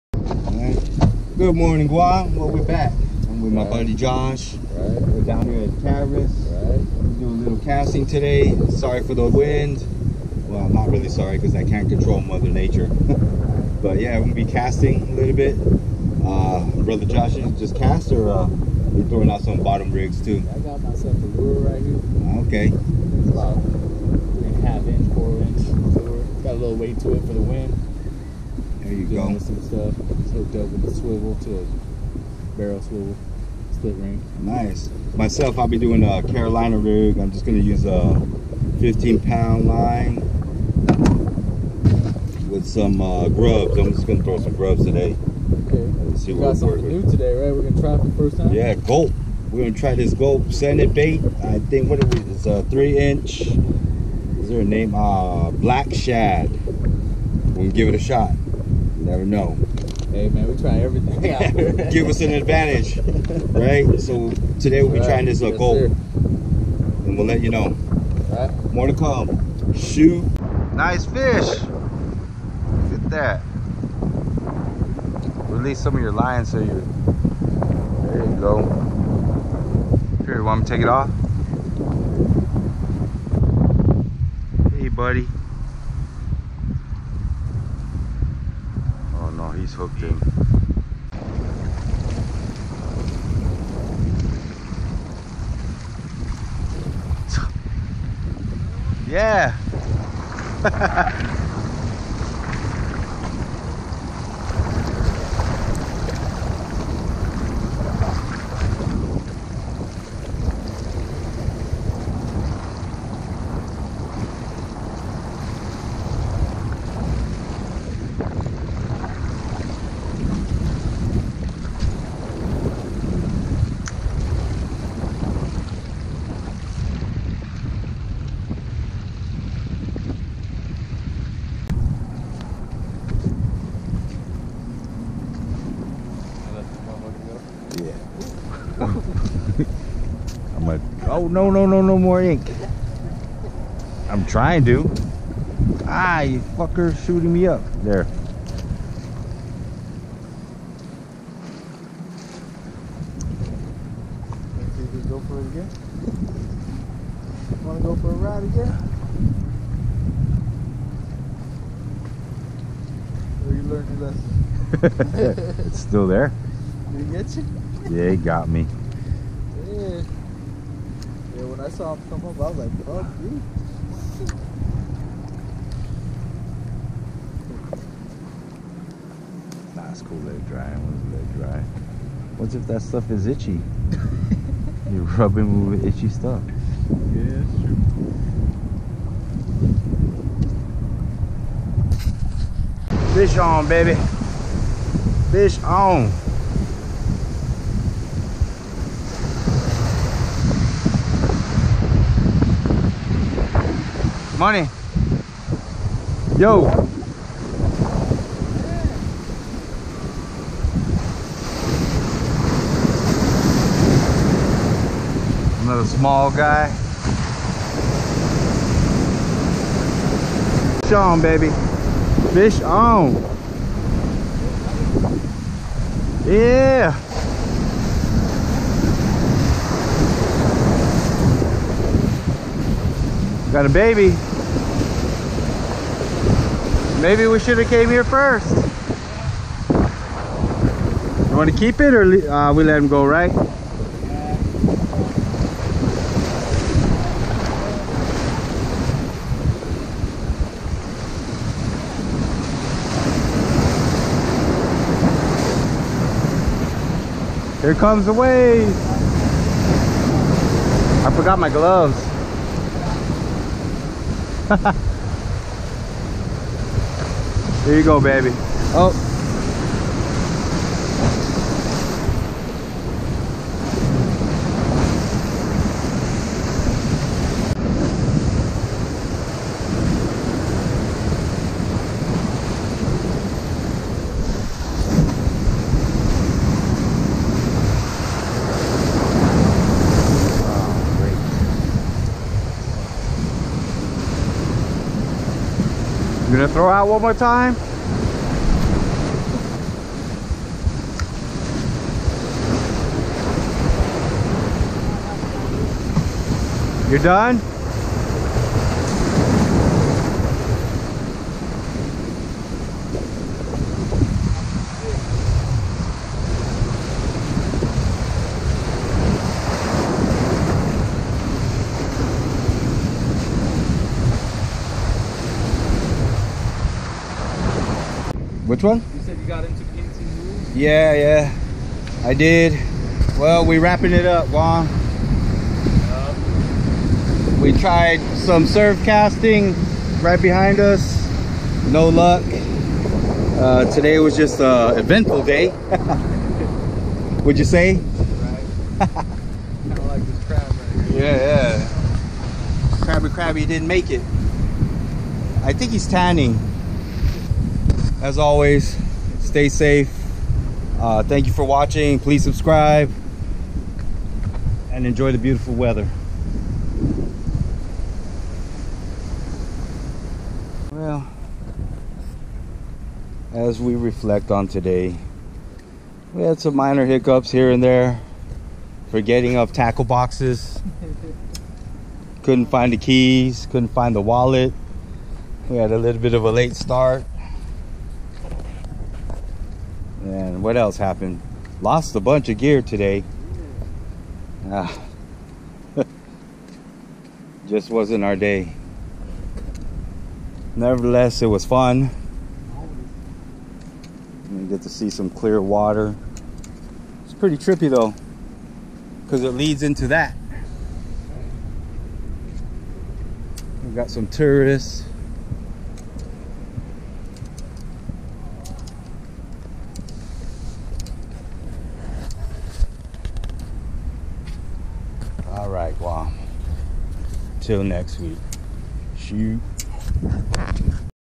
Casting.Surf
Various styles of baits being used. All casting..
Thanks for the eyeballs. Catch it don't buy it..